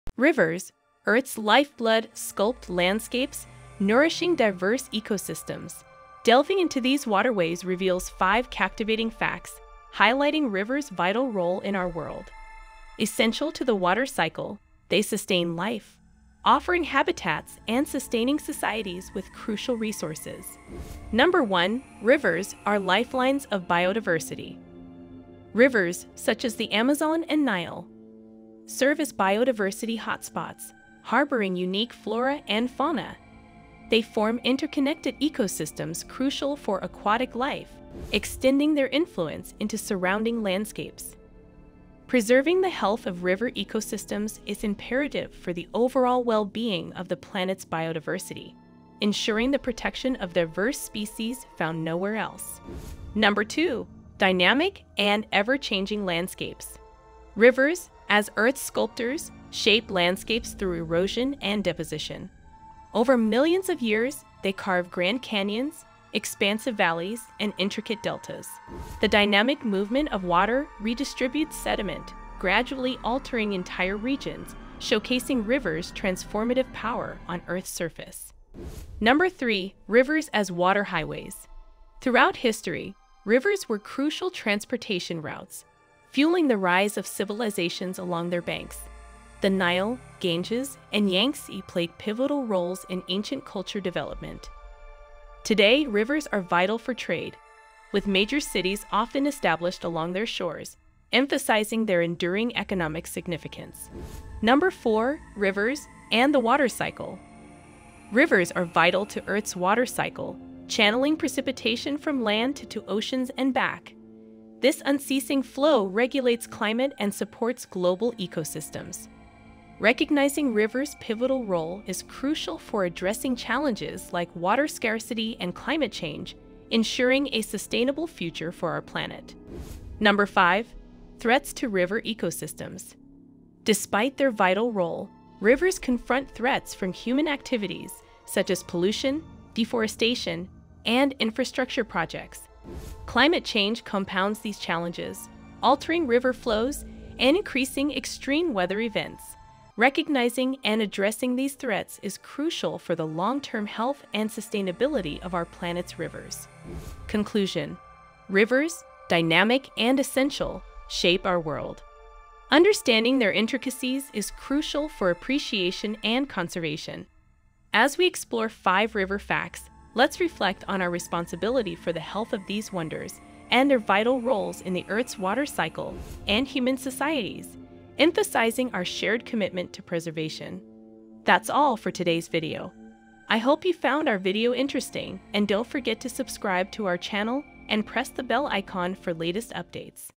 Rivers Unveiled: Fascinating Facts and Wonders of Earth's Lifelines!
Embark on a mesmerizing journey with us as we uncover the untold stories of rivers, the lifeblood of our planet. From their role in shaping landscapes to their cultural significance, join the adventure of discovery with these top 5 facts about rivers!

🔍 Video Highlights:

🌿 Ecosystem Marvels: Dive into the diverse ecosystems supported by rivers, from mountain streams teeming with trout to expansive deltas hosting a rich array of aquatic life.

🌎 Geological Architects: Witness the transformative power of rivers as they sculpt the Earth's surface, carving canyons, shaping valleys, and depositing sediments to create fertile plains.

🌐 Cultural Tapestry: Explore the cultural importance of rivers throughout history, from ancient civilizations that thrived along their banks to modern cities that depend on these waterways for commerce and transportation.

💦 Water Cycle Connection: Understand the integral role rivers play in the water cycle, acting as conduits that transport water from land to the oceans, influencing weather patterns and sustaining life.

🚣‍♂️ Recreation and Exploration: Join us on a journey of recreation and exploration along some of the world's most iconic rivers, showcasing how these waterways provide opportunities for adventure and leisure.